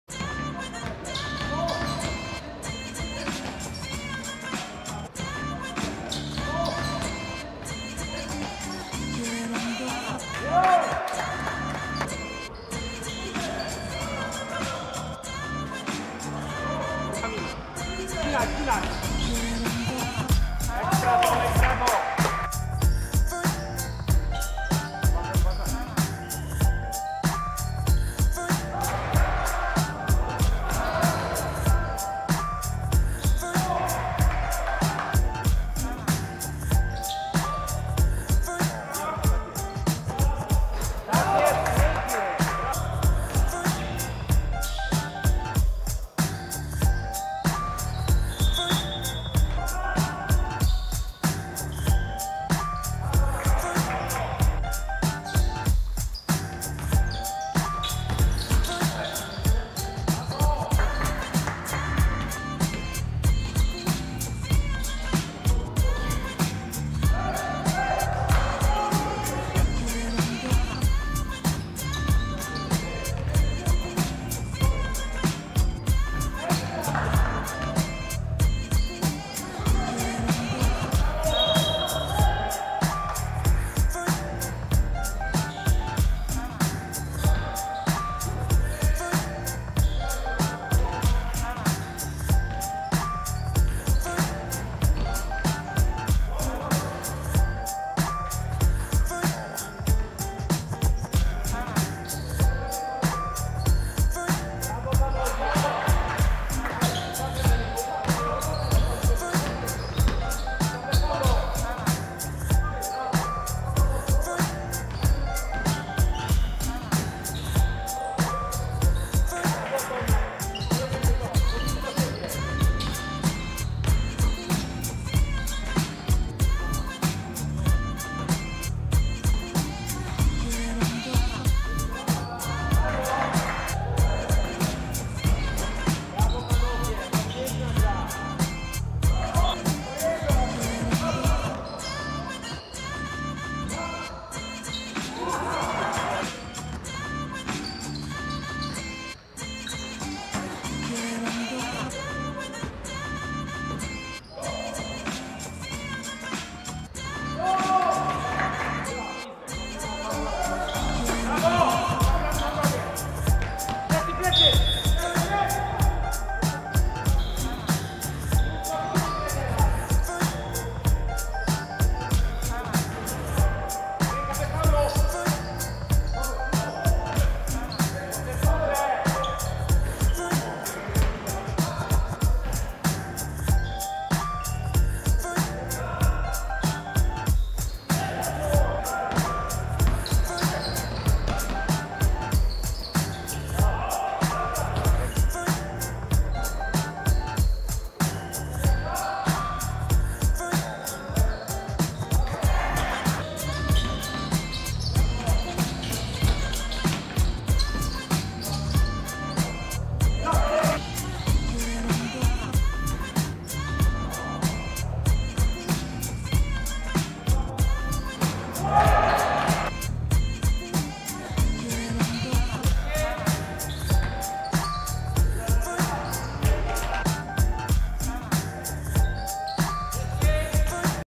04 10 Njersey 3u vs New York 3u Basket 36+
NJERSEY 3U 54:49 NEW YORK 3U
T.Gerung 16pkt 6zb
Ł.Karaszkiewicz 14pkt 7zb